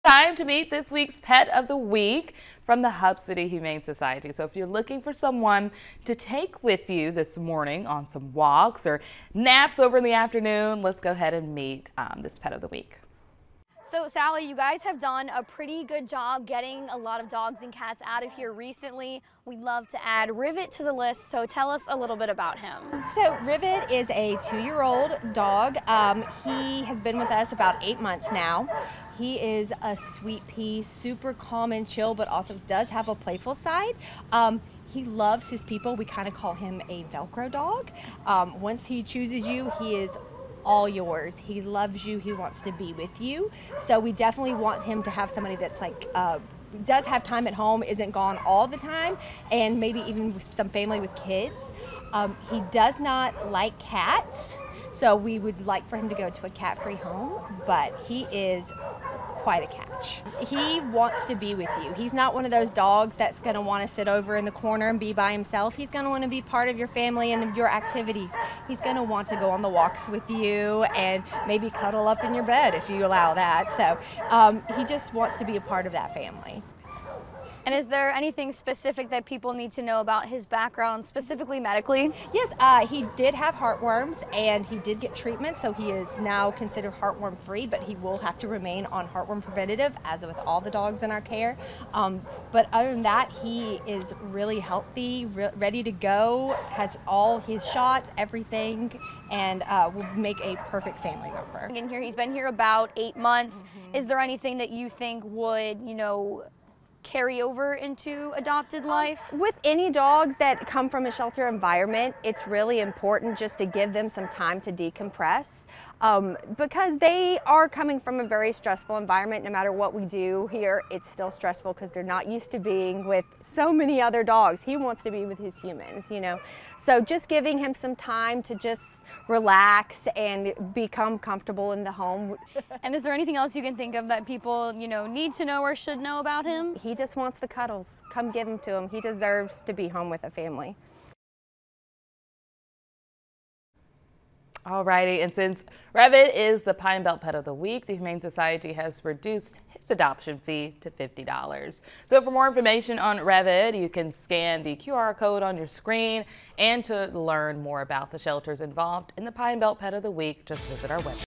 Pet of the Week: Rivet
Meet Rivet, this week’s Pine Belt Pet of the Week from the Hub City Humane Society.